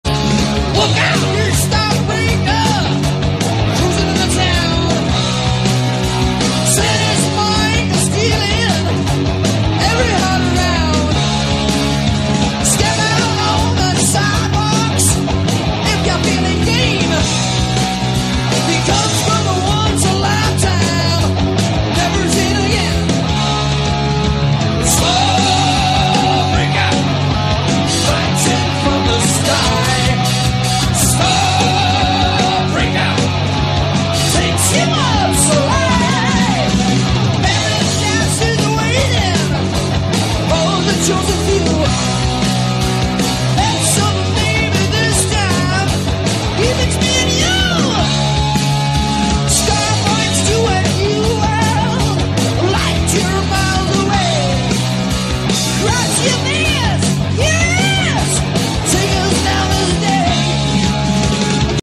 Top 10 SeXy, HaWt, gAy pics of Rob Halford of Judas Priest
Heavy Metal Picker has legions of (mostly) female fans who wanted the hottest pics of Rob Halford. Well, here you go!